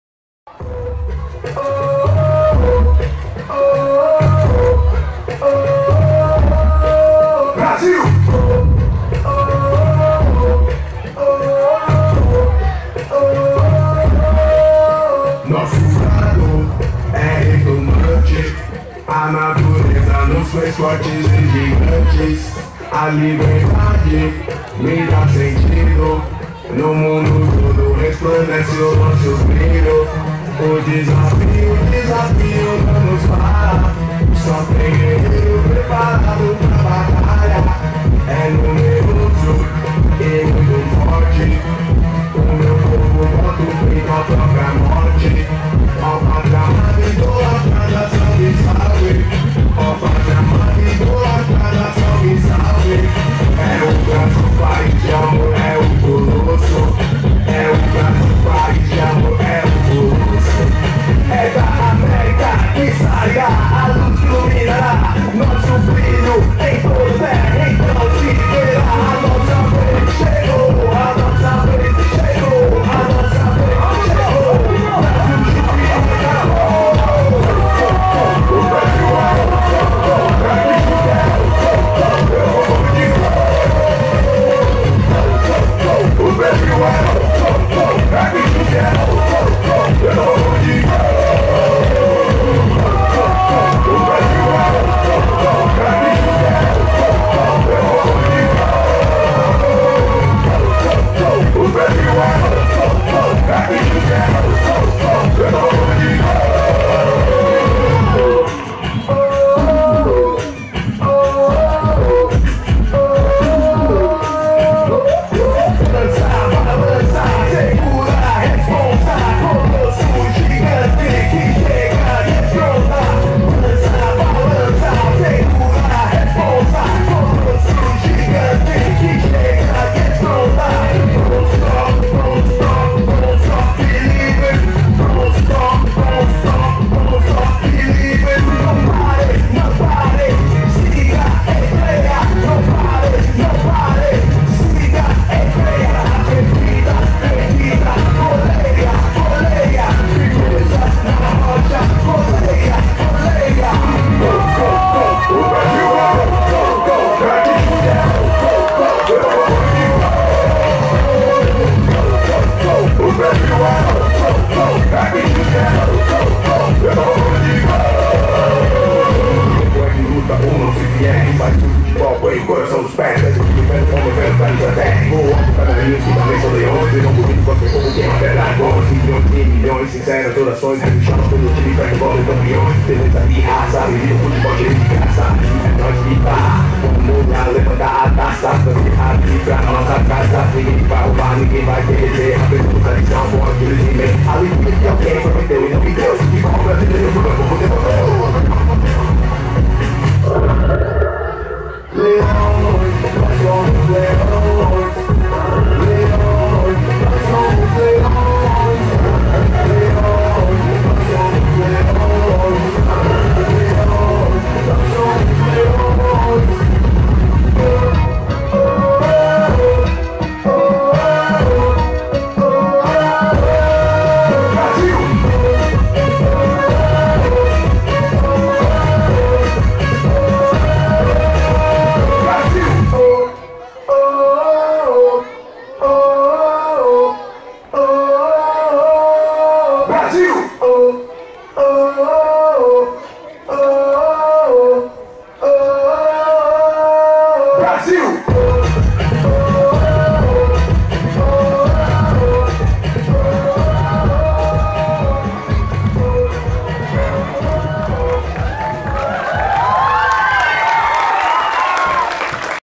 14ª Mostra Prof. José Lutzenberger (2015) - Coração Brasileiro
Apresentação da EM Maria Ferreira Couto na 14ª Mostra Prof. José Lutzenberger - "Escola Amiga do Meio Ambiente" (Mostra Lutz) realizada em 20 de novembro de 2015 em Garopaba (SC).